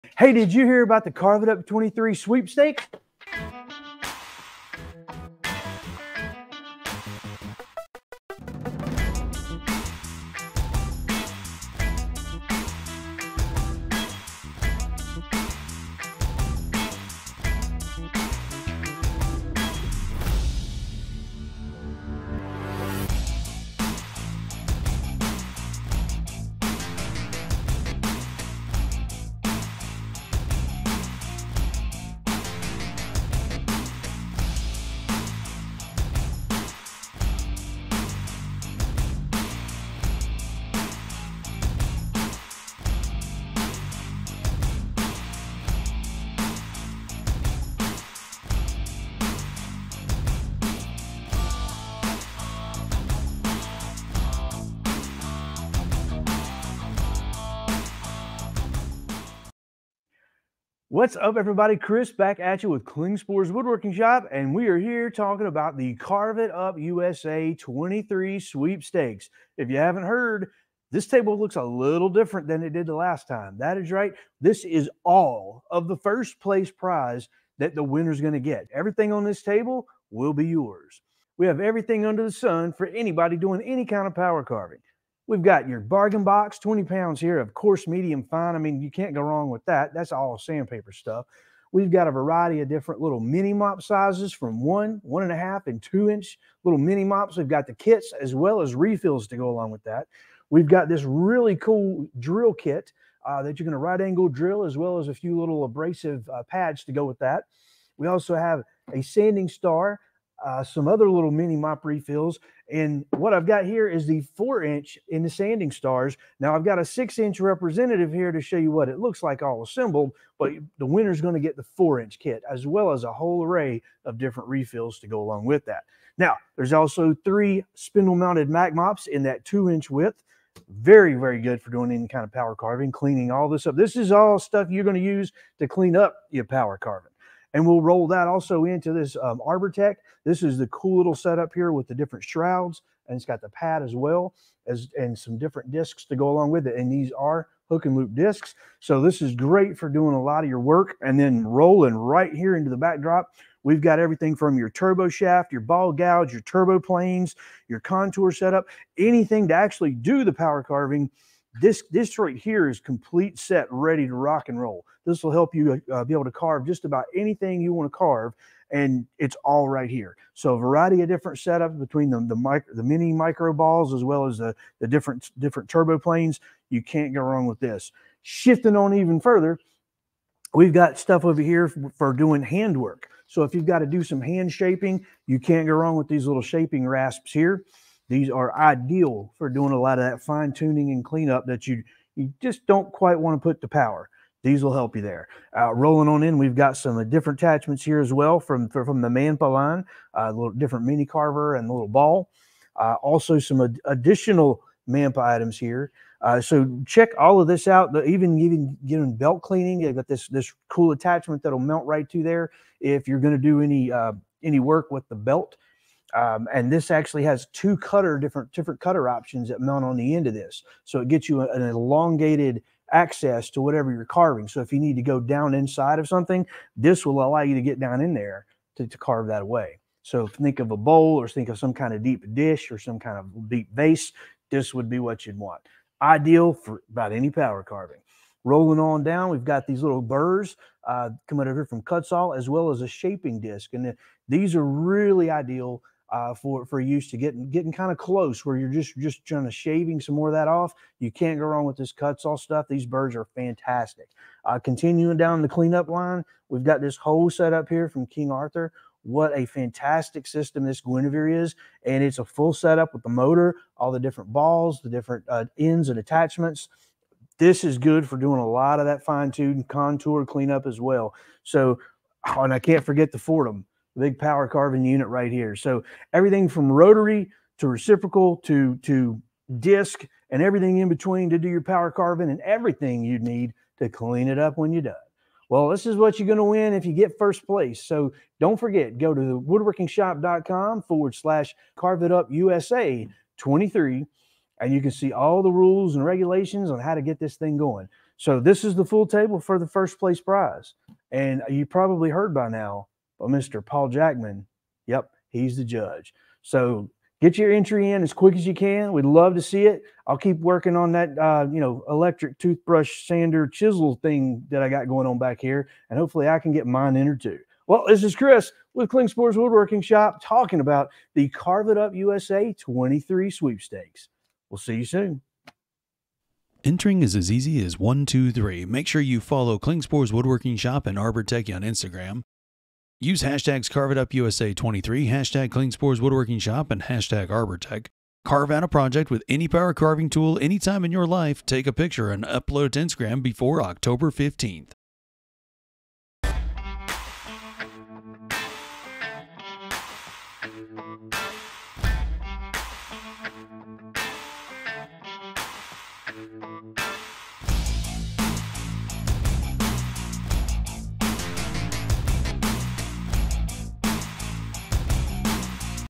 Carve It Up USA 23
follow @klingsporswoodworkingshop and @arbortechie on Instagram
Carve out a project with any power carving tool, take a picture, and upload it to Instagram before Oct 15th 2023.

Snap It
Take a photo of your project. Past, present, or current work, doesn't matter! We want to see your power carving skills.

Tag It
Use the carveitupusa23 klingsporswoodworkingshop arbortech hashtags and upload them to Instagram.

Win It
Three Prizes Up For Grabs! Your entry could be the winning one!

Remember, you must use a power carving tool with your submission. Let's see that work!